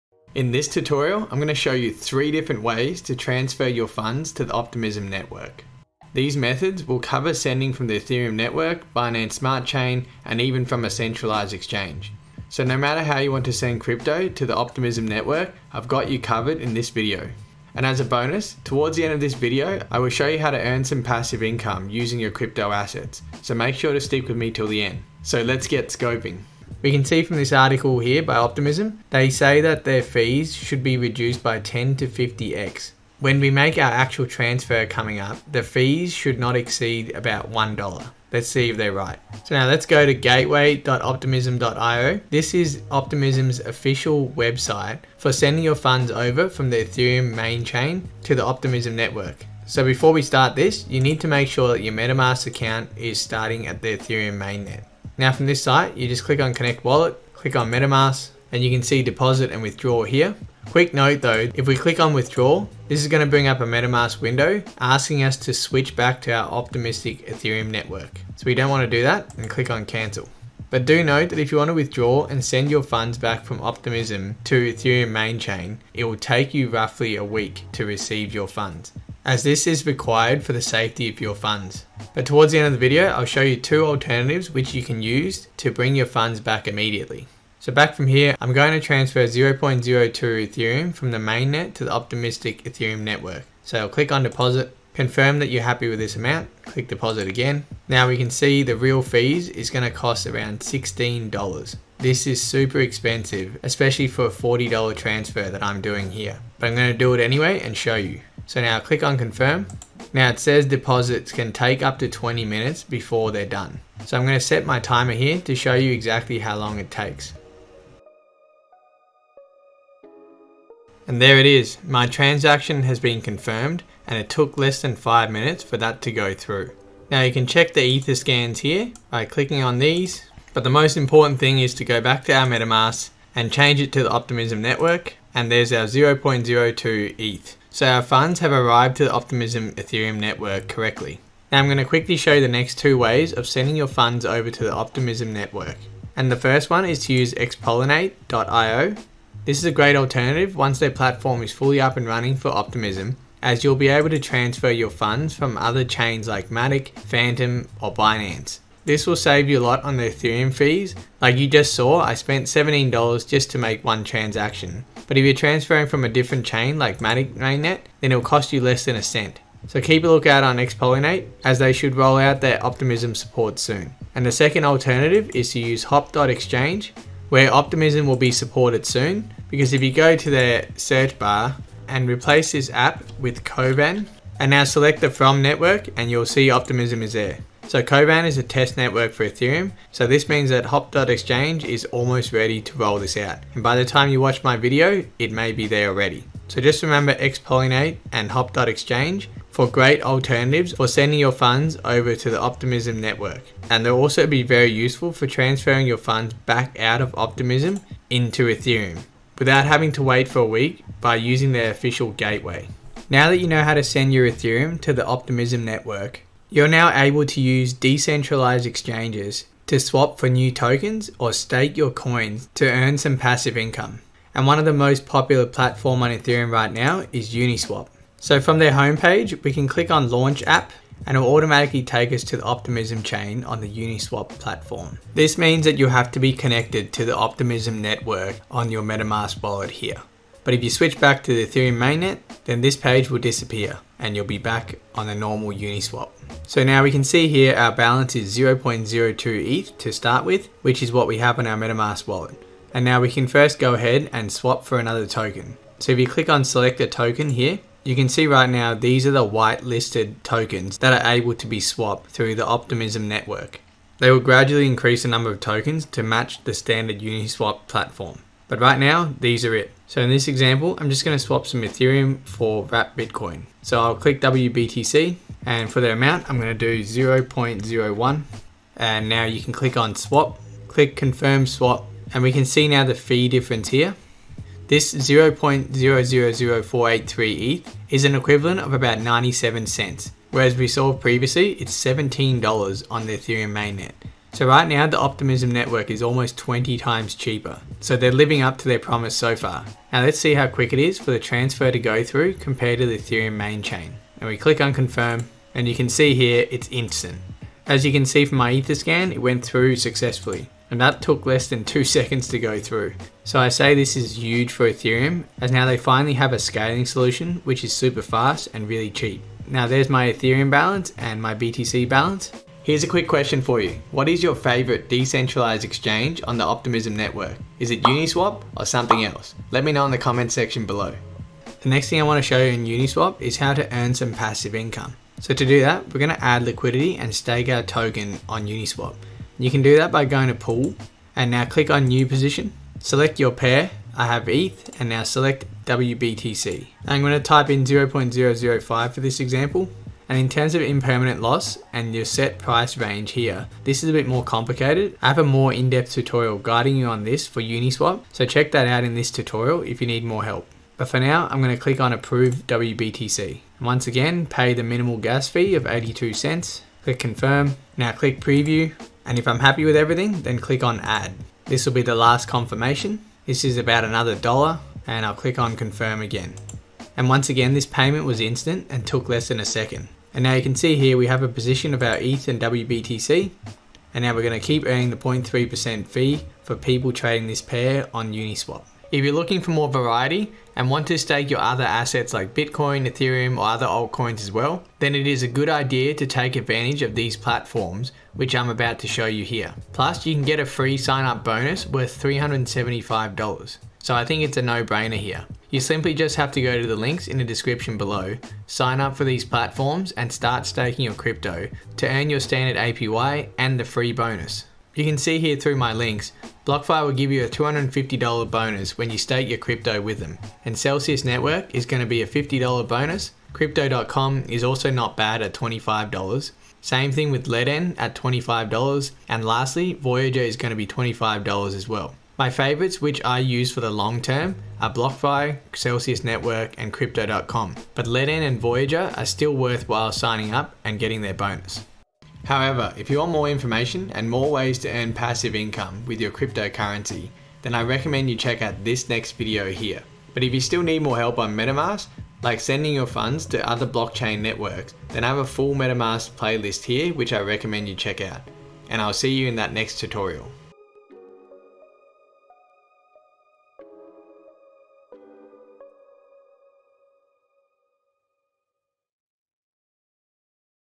How To Transfer Funds To Optimism Network ✔️(Send Crypto From Ethereum, Binance Or Matic/Polygon)
In this tutorial, I'm going to show you 3 different ways to transfer funds to Optimism Network. These methods will cover sending from the Ethereum Network, Binance Smart Chain and even from a centralized exchange. So no matter how you want to send your crypto to the Optimism Network, I’ve got you covered in this video.

📕 FREE CRYPTO TRADING CALCULATOR 📕 keep track of your investment and trading profits

⏱️TIMESTAMPS⏱️
00:00 - Overview & Bonus
00:39 - Send Funds From Ethereum Mainnet To Optimism Ethereum With Gateway.optimism.io 02:26 - Option 1 To Send Funds With Xpollinate
02:55 - Option 2 To Send Funds With Hop.Exchange
03:44 - Swap Token & Stake Crypto With Uniswap.Org
05:36 - Earn Passive Income On Your Crypto